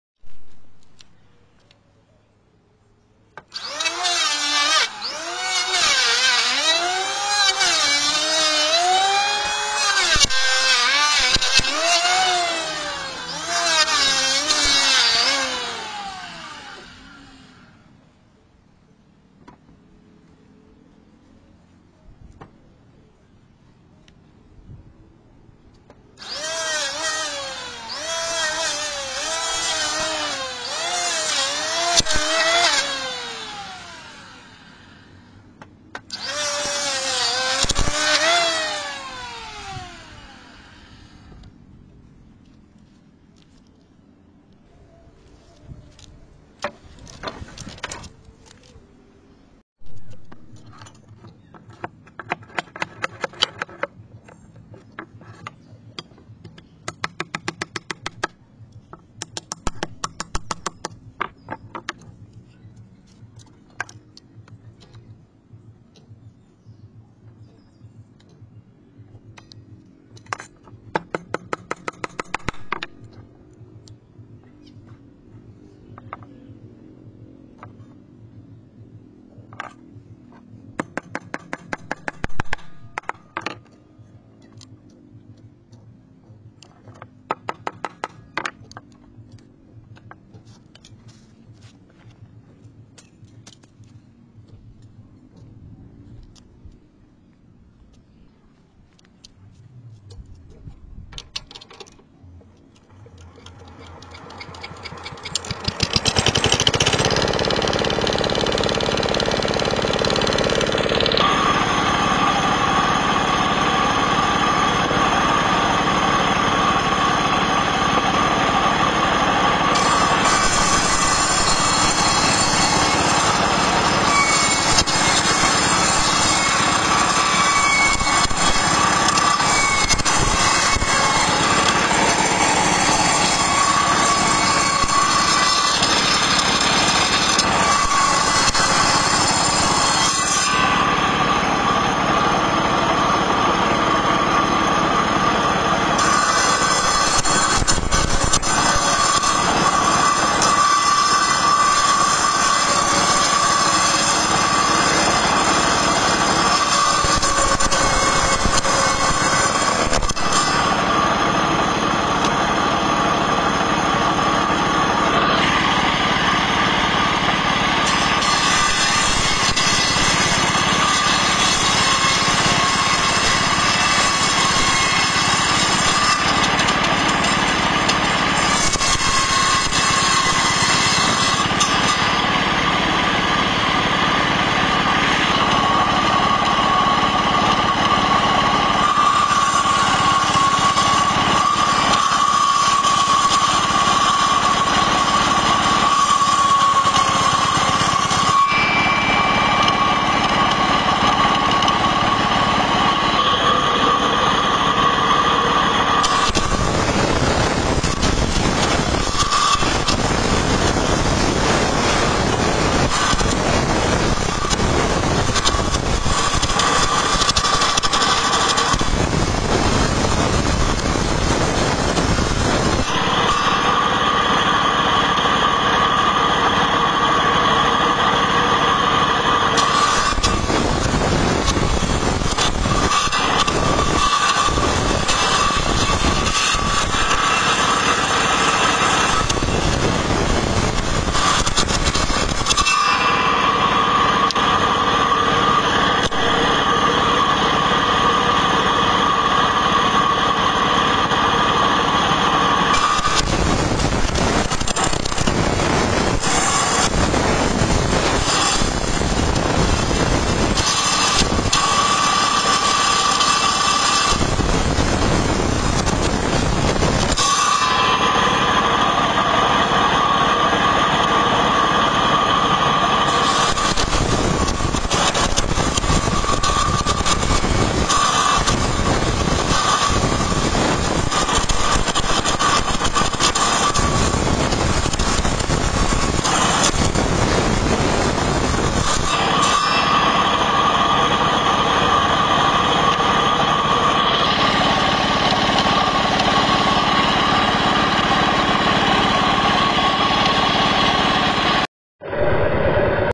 របៀបជ្រៀកឈើ How to cut wood
This video wants to show you how to make a bed, make a cupboard, make and carve a luxury wood carving. If you like my video, please help me by clicking on one.របៀបជ្រៀកឈើ How to cut wood